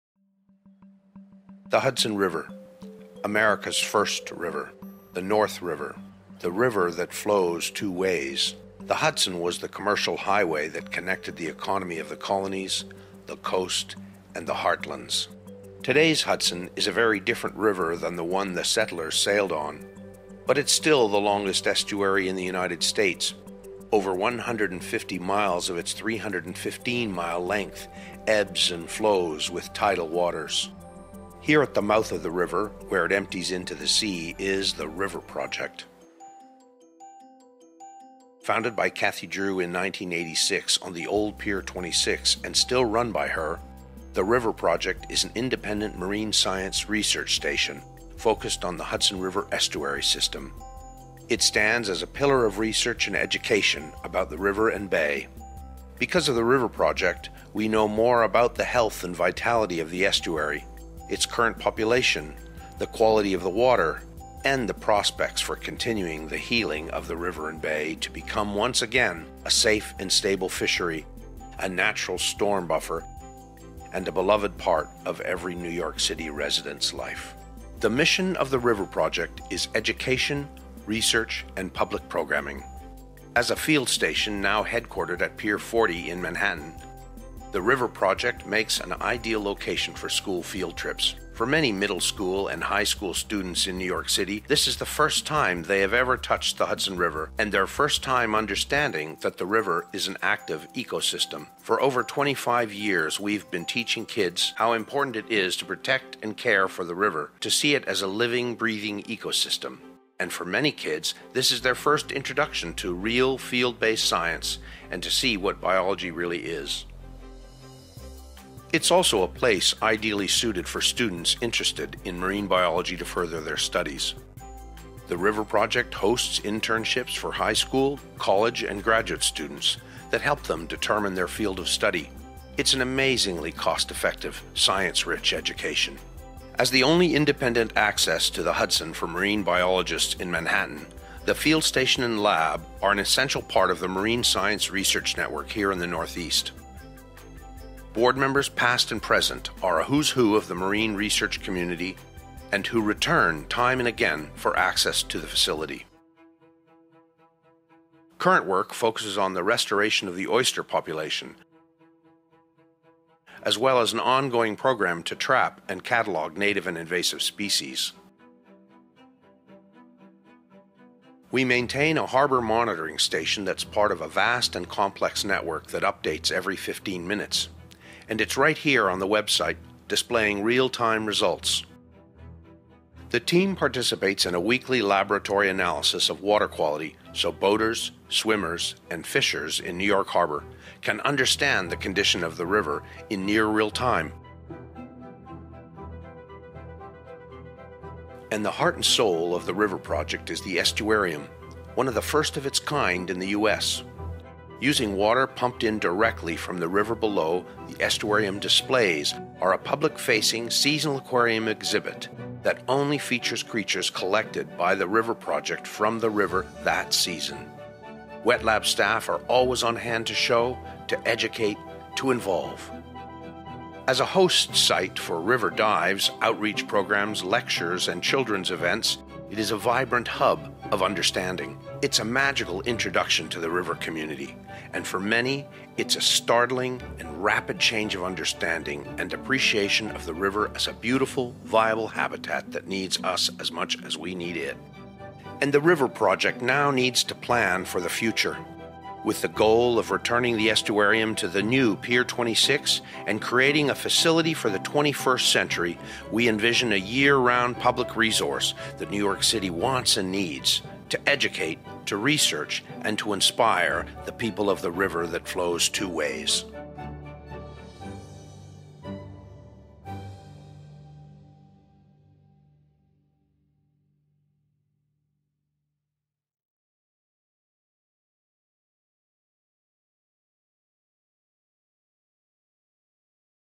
Meet The River Project
The River Project is a marine science field station and environmental education center operating on Pier 40 on the Hudson River Estuary in Lower Manhattan. Watch this video to learn about our history, accomplishments, and current programs. Special thanks to David Fink and Marc Bachmann for the creation of the video.

Please consider donating to The River Project by following this